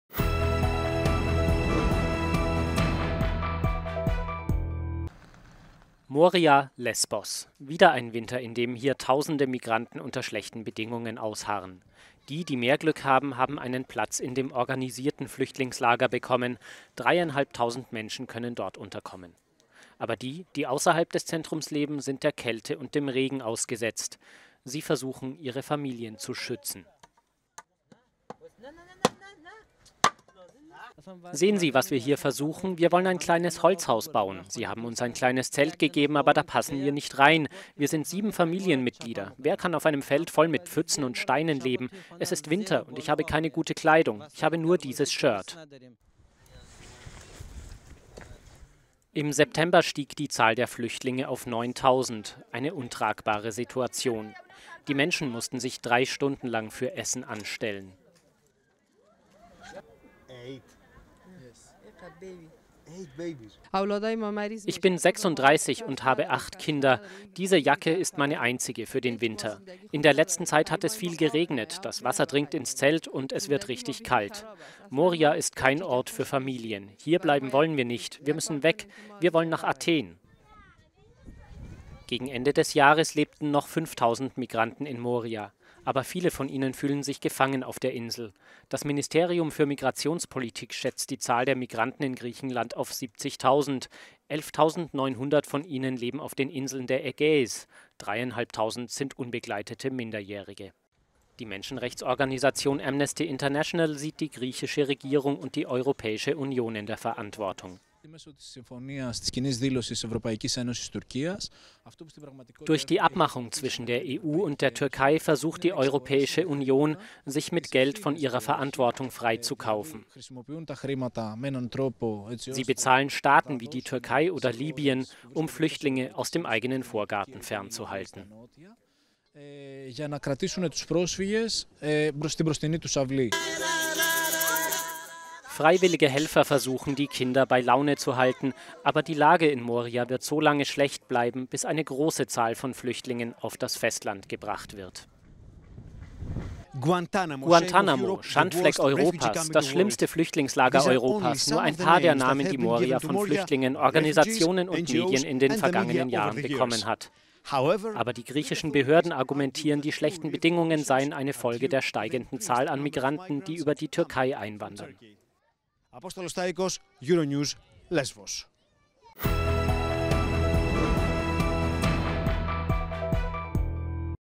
Lesbos: Kaltes, nasses "Guantanamo" - ein weiteres Jahr im Flüchtlingslager
Auf der griechischen Insel Lesbos liegt das Flüchtlingslager Moria. Es ist hoffnungslos überfüllt. Die Politik schiebt anderen die Verantwortung zu.…